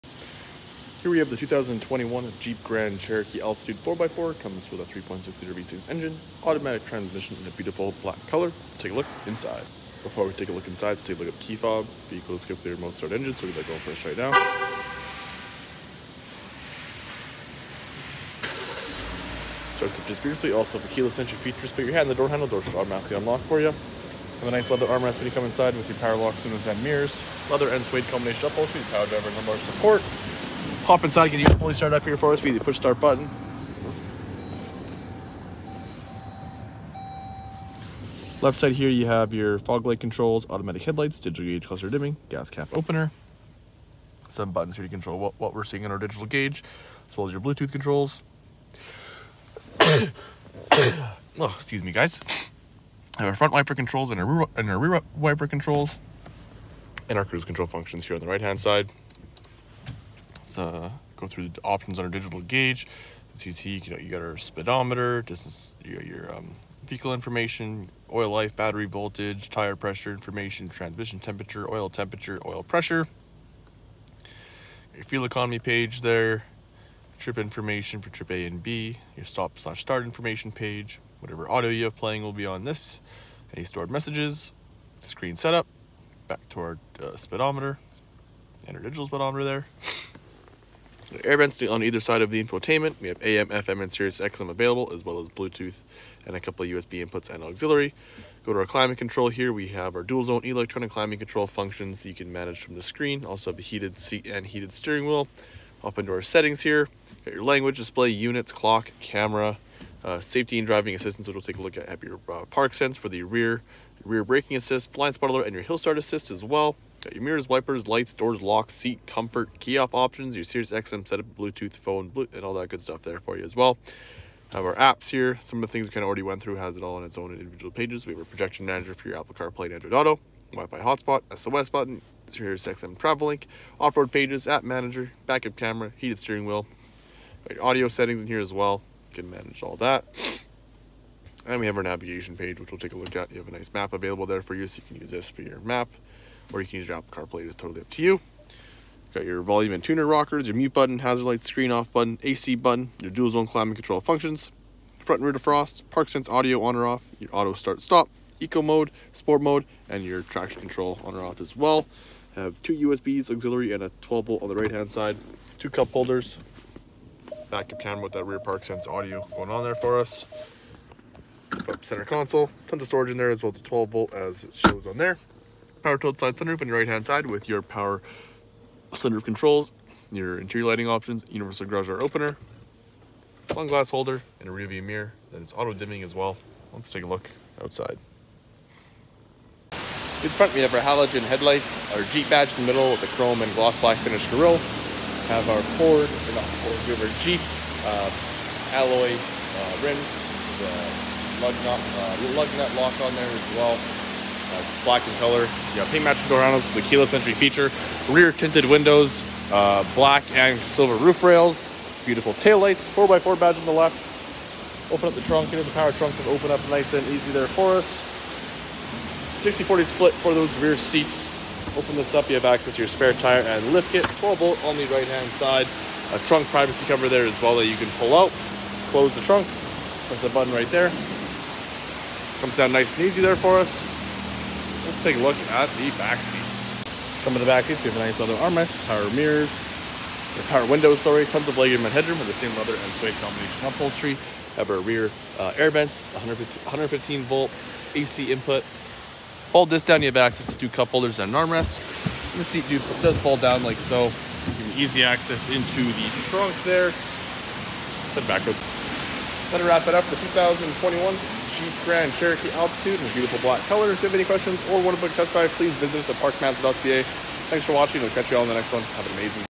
2021 Jeep Grand Cherokee Altitude 4X4 Review - Park Mazda
Stock #218865

Rise above daily driving in the 2021 Jeep Grand Cherokee Altitude 4X4 that's dynamic and distinctive in Diamond Black Crystal Pearl! Powered by a 3.6 Liter Pentastar V6 delivering 295hp matched with an 8 Speed Automatic transmission that's as eager for adventure as you are. This Four Wheel Drive SUV boasts authentic off-road performance with Selec-Terrain traction management and it scores nearly approximately 9.4L/100km on the highway. The iconic good looks of this Grand Cherokee shine through with Halogen Headlamps, LED lighting, a power tailgate, a dual-pane sunroof, Granite Crystal accents, and alloy wheels.